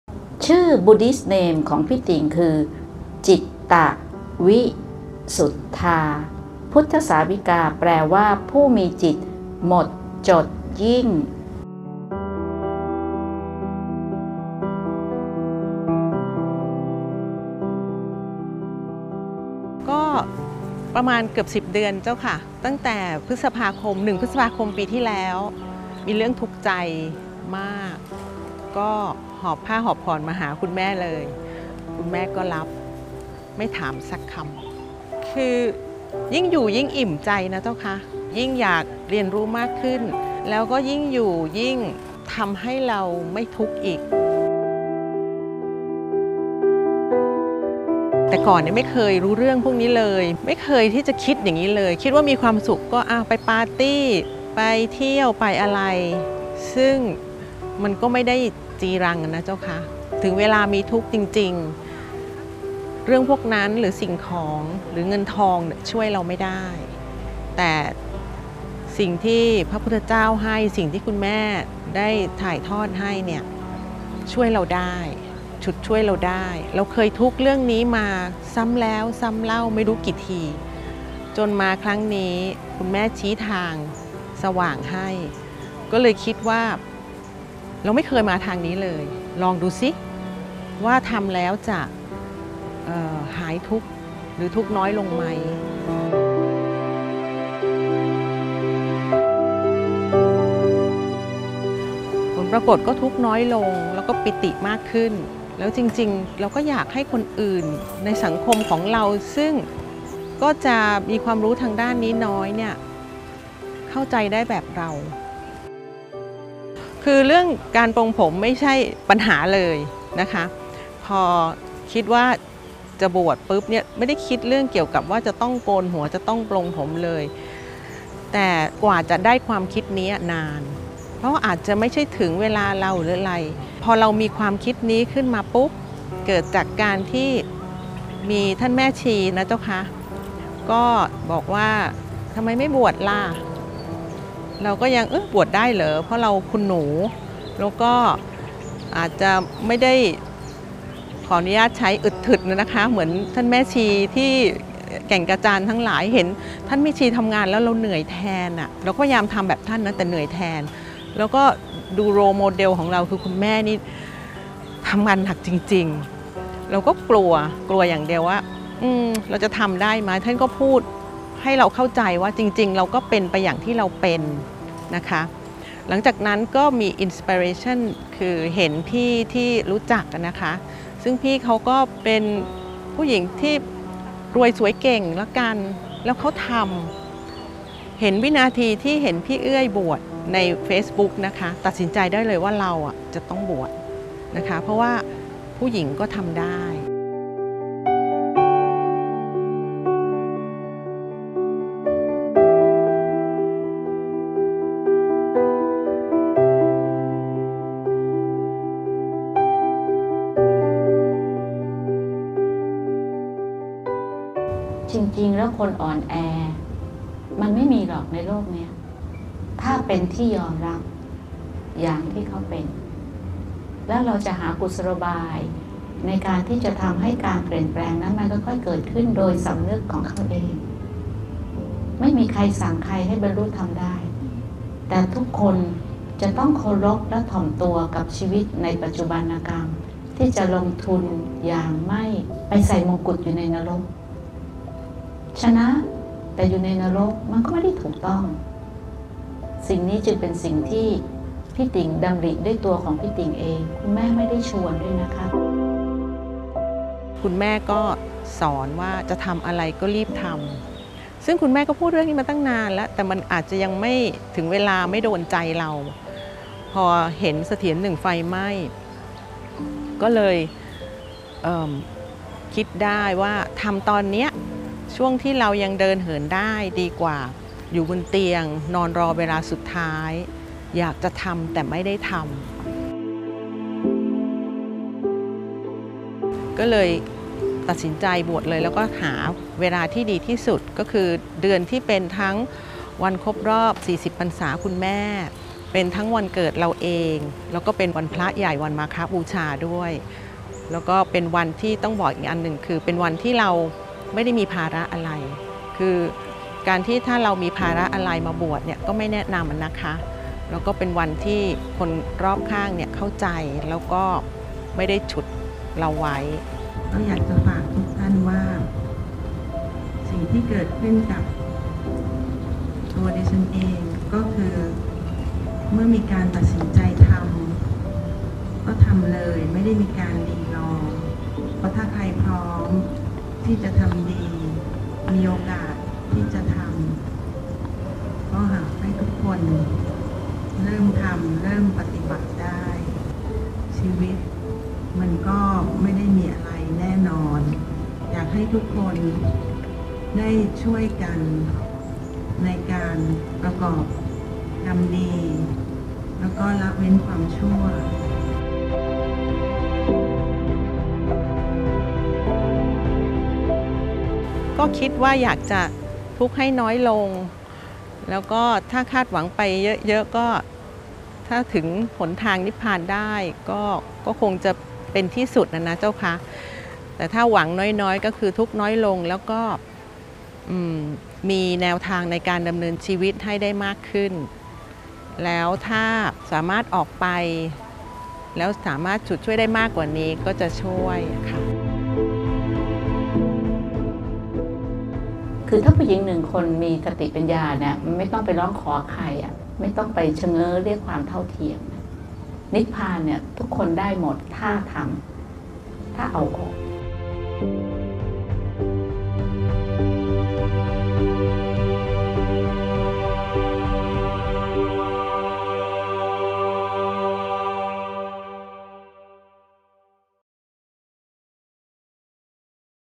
จิตตะวิสุทธา พุทธสาวิกาผู้มีจิตหมดจดยิ่ง (แม่ชีติ่ง ปรารถนา มงคลกุล)
ไม่มีใครสั่งใครให้บรรลุธรรมได้
ชนะ...แต่อยู่ในนรก ก็ไม่ถูกต้อง

นิพพานทุกคนได้หมด...ถ้าทำ
และเอาออก
ทิ้งทั้งหมด...ได้ทั้งหมด

ทำตอนนี้ในวันที่เรายังเดินได้
ตัดสินใจบวช
วันนี้ทุกข์น้อยลง ปิติมากขึ้น
หวังน้อย ก็ทุกข์น้อย
มีแนวทางในการดำเนินชีวิต
และออกไปฉุดช่วยคนได้มากขึ้น

แม่ชีติ่ง ปรารถนา มงคลกุล
เสาร์ที่ 10 เมษายน 2564
ขอเชิญผู้สนใจร่วมโครงการบวชพุทธสาวิกา ณ เสถียรธรรมสถาน

ขอรับบวชพุทธสาวิกาศีล 10 ได้ที่ไลน์ @sdsnun